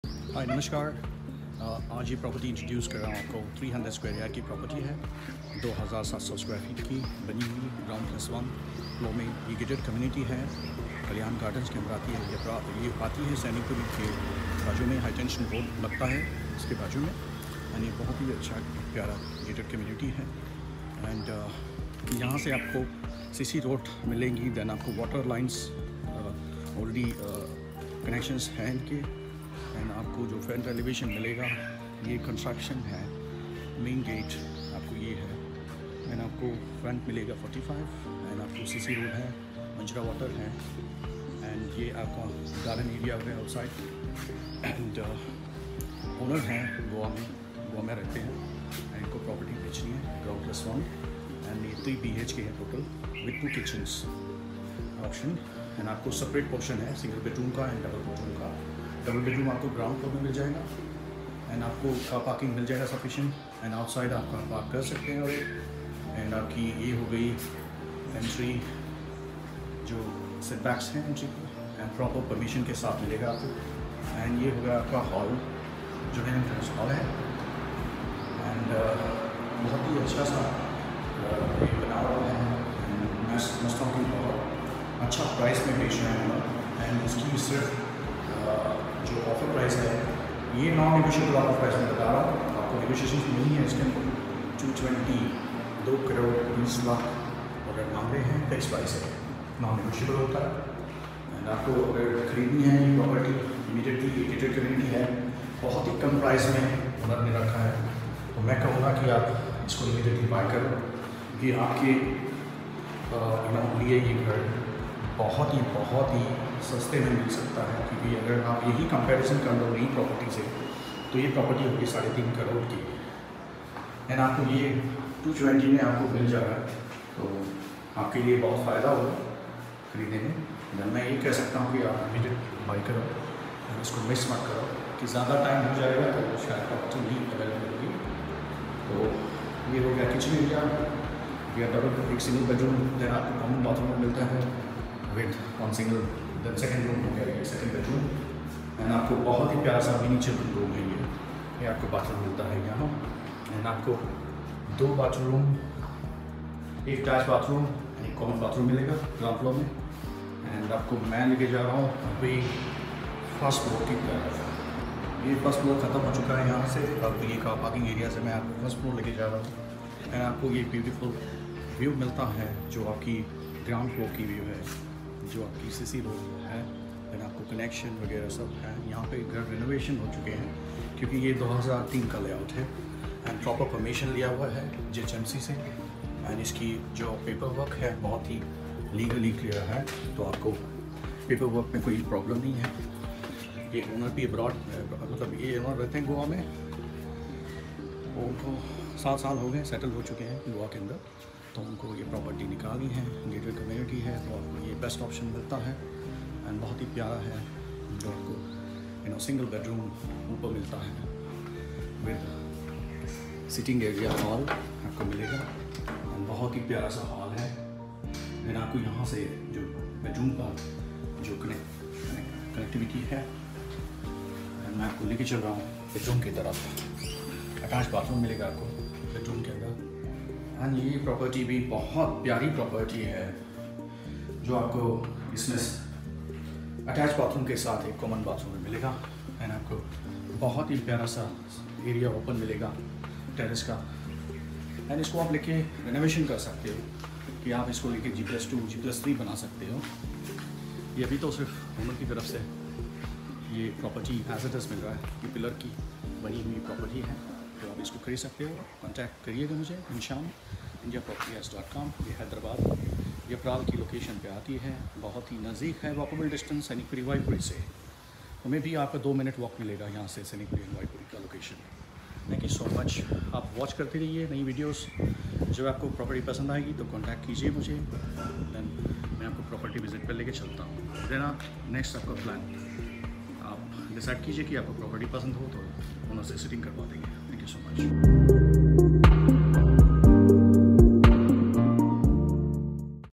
"House for Sale in near Sainikpuri Society - 3BHK West Facing Property - 2 Crores 20 Lakhs"
Are you looking for a new home in Sainikpuri Society? Look no further! This beautiful 3BHK property is located near Kalyan Gardens gated community, offering a spacious 2900 square feet of construction area.

With a west-facing orientation, this property boasts a car parking space and a garden area for your enjoyment. Both manjeera water and bore water are available, ensuring a reliable water supply.

The owner is asking for a non-negotiable price of 2 crores 20 lakhs. Don't miss out on this fantastic opportunity to own a piece of paradise in Sainikpuri Society.